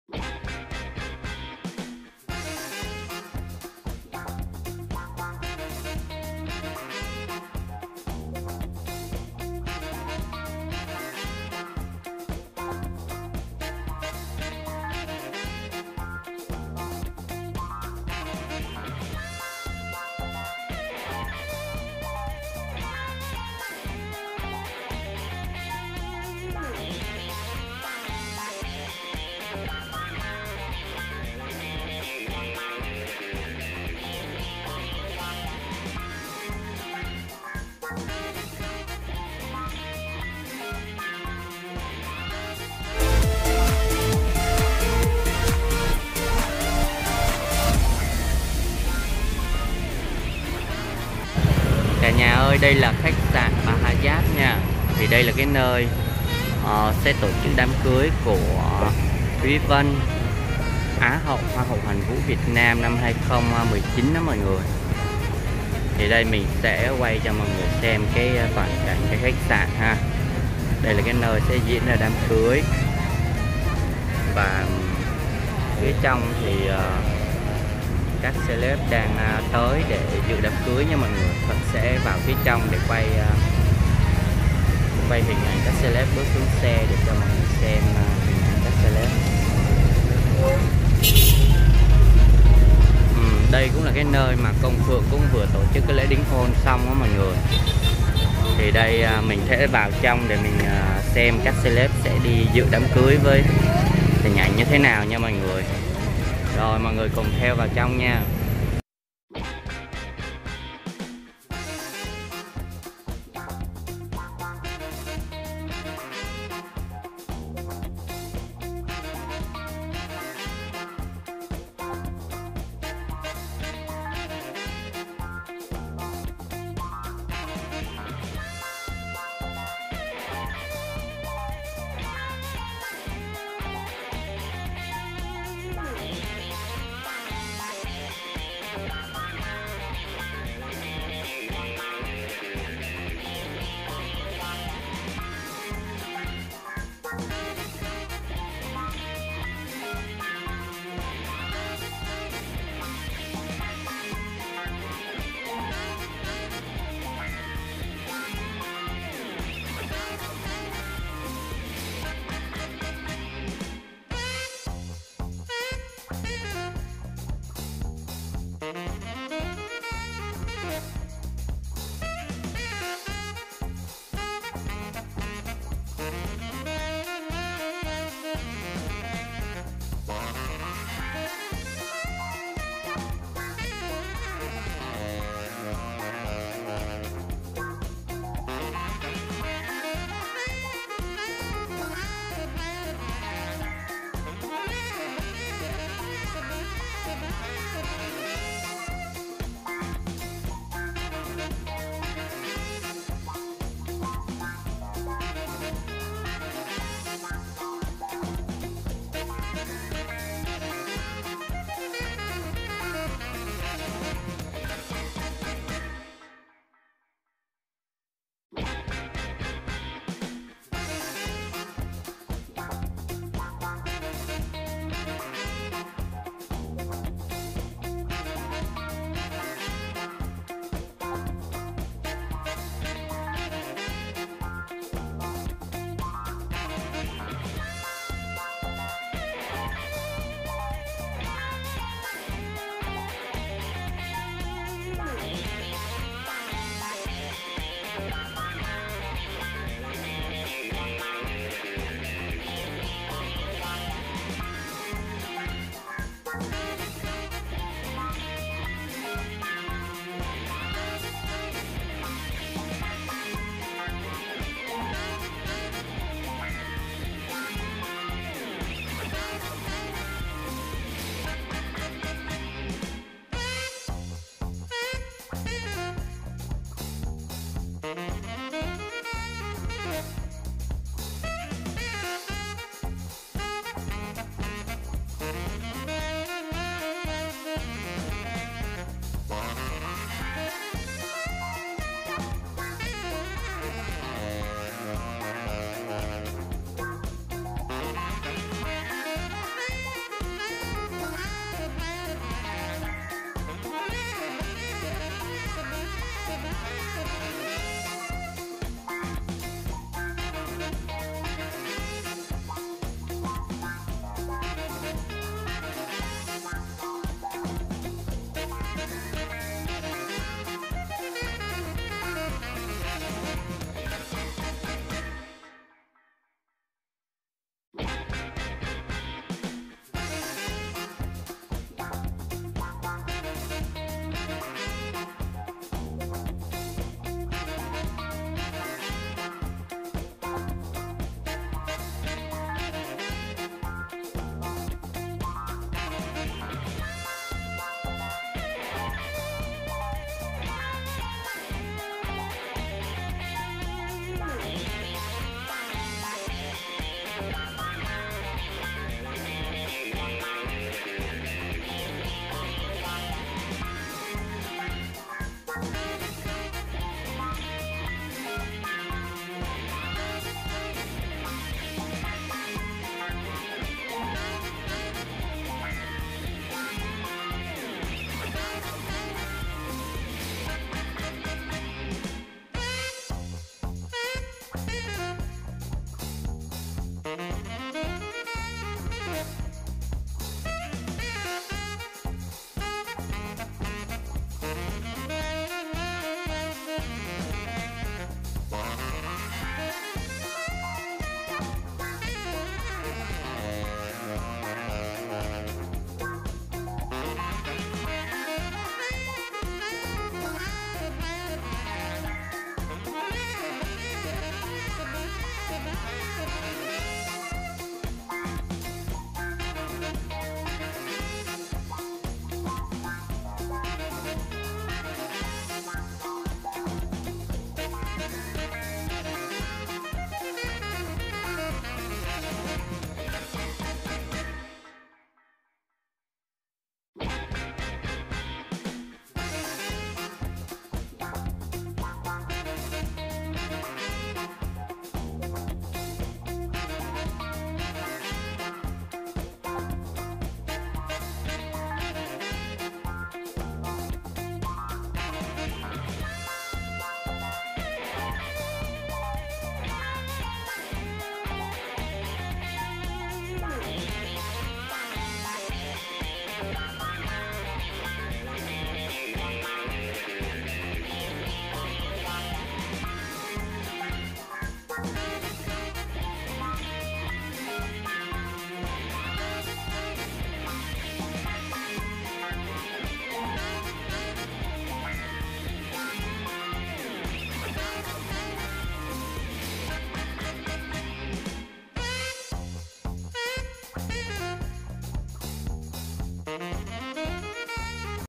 Hoa hậu HƯƠNG GIANG, H'HEN NIÊ lên đồ lộng lẫy dự đám cưới THÚY VÂN
Hoa hậu Hương Giang và H'Hen Niê lộng lẫy tại đám cưới Thúy Vân.

Kênh SaigonDaily chuyên về giải trí showbiz, Lô tô show và đời sống với thông tin được cập nhật liên tục, mỗi ngày.
©️ Bản quyền thuộc về Saigondaily ©️ Copyright by Saigondaily. DO NOT REUP!